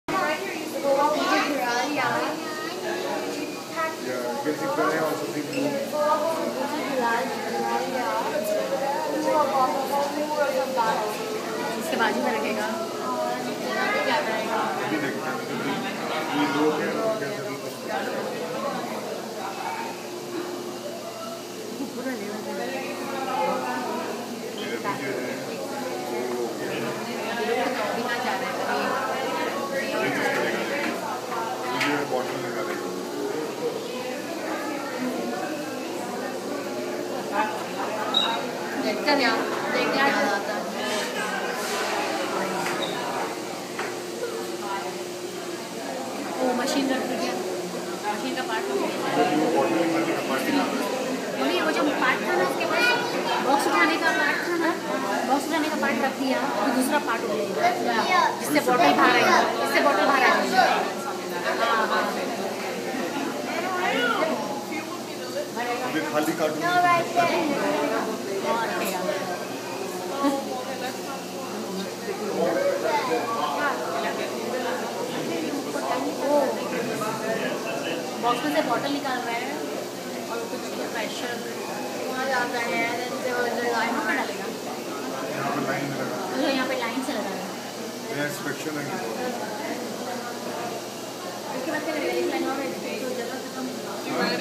Secret of Coca Cola | Coke factory tour 2012
I took this video with my iPhone 4s during my Coca Cola factory tour in Atlanta, Georgia with my friends and family. This shows the demo robot busy doing part of the packaging work. There are tons of attractions inside the factory besides this and I highly recommend it to anyone who wants to discover the secret of Coke. Coca Cola is proud to be one of the biggest sponsors of the 2012 London Summer Olympics. Coca Cola open happiness!